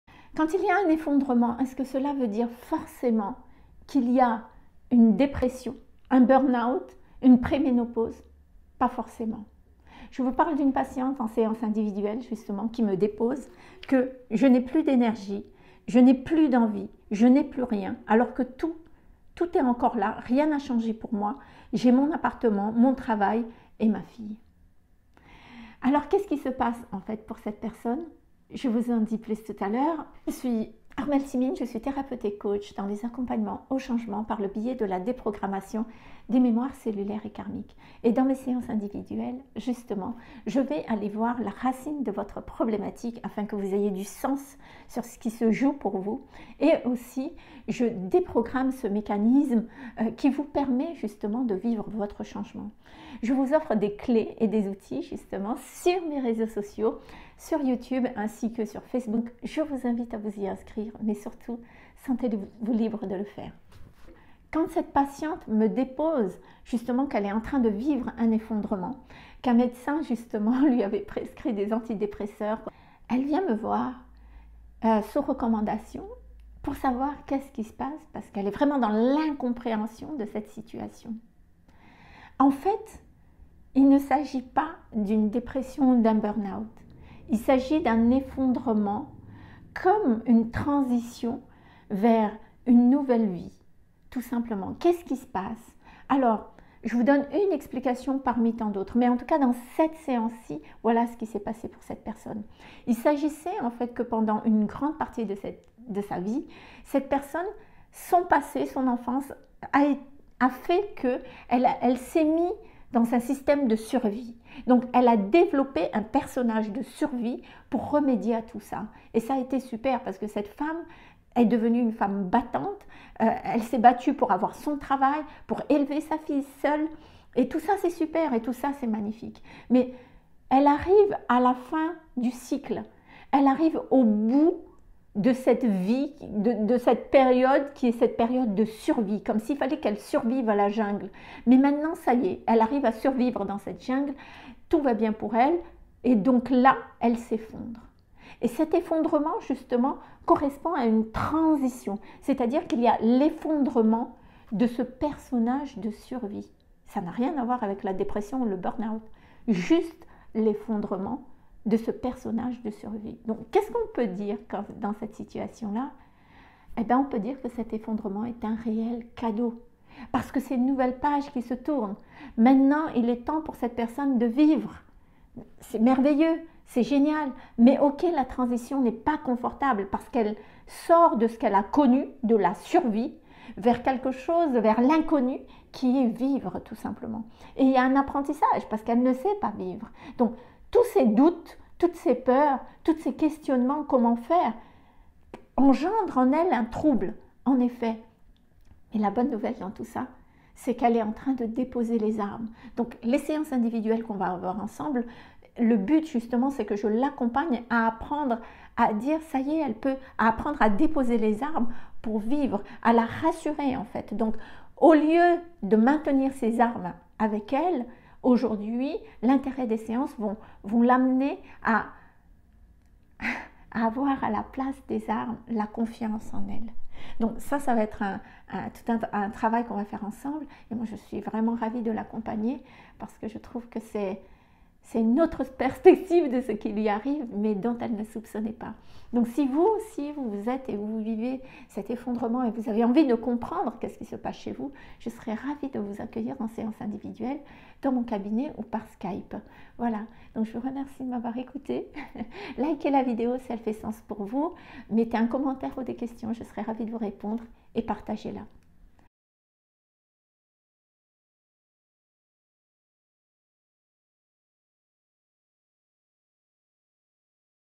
Un effondrement peut être un cadeau
⬇️ Déroulez la description ⬇️

Quand il y a un effondrement est-ce que cela veut dire forcément qu’il y a une dépression ou un burn out ? et bien non, pas forcément …

Cet effondrement peut cacher un magnifique cadeau !

                                  🔺 QUI SUIS-JE ? 🔻

                                 🔺 MON APPROCHE 🔻

Je suis spécialisée dans :
🔹 La lecture des blocages inconscientes :

             🔺ACCOMPAGNEMENT INDIVIDUEL 🔻

                                🔺 MON RÔLE 🔻

Armelle Tsimine